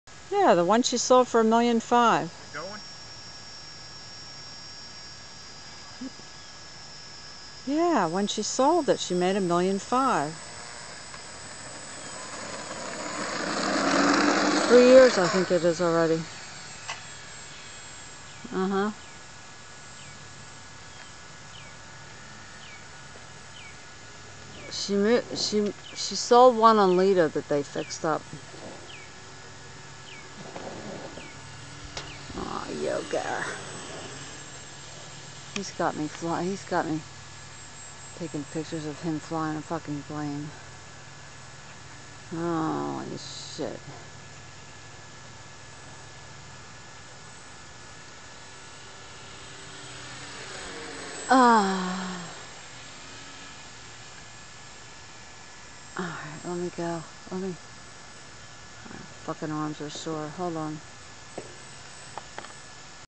FLITE WITH MY NEW TROJAN T-28. AND SEWER MOUTH EX GIRFRIEND FILMING A WHOLE MINUTES WORTH.
JUST BOUGHT THIS TROJAN T-28 LAST WEEK,HAVE BEEN FLYING A MONTH.